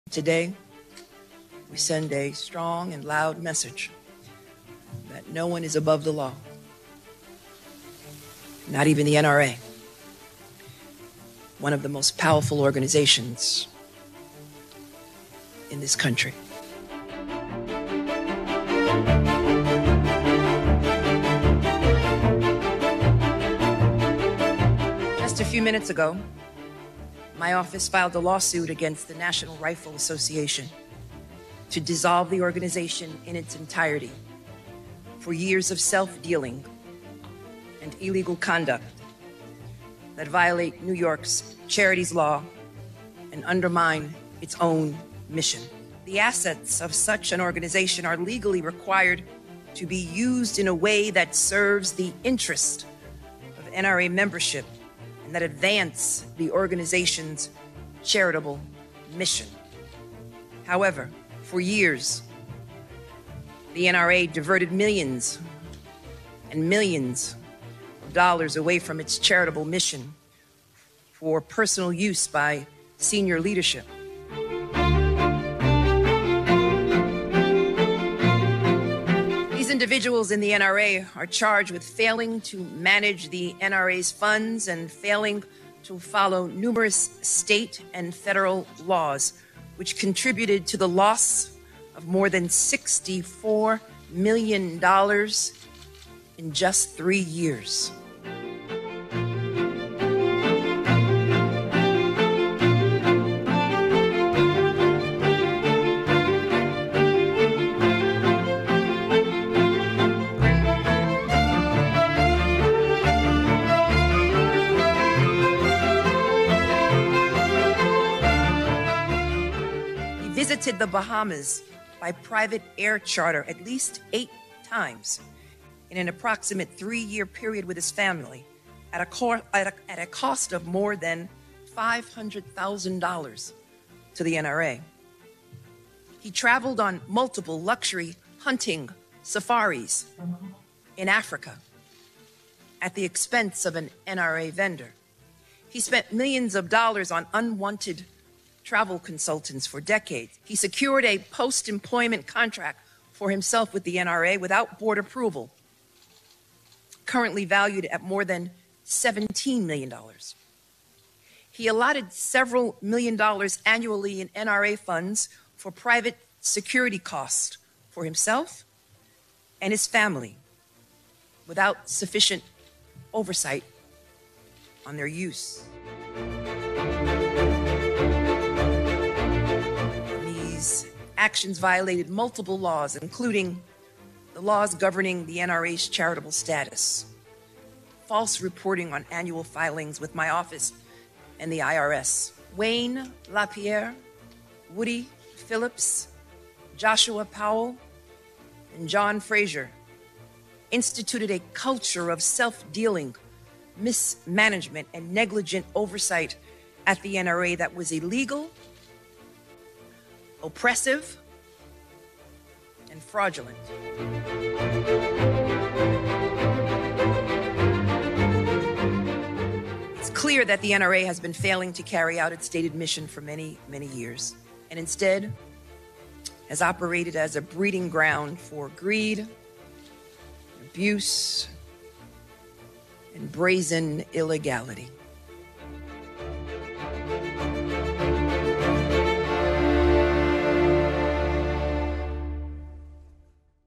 NY AG Letitia James Files Suit to Dissolve NRA | NowThis
‘No one is above the law, not even the NRA’ — NY AG Letitia James is suing to dissolve the NRA over ‘years of self-dealing and illegal conduct.’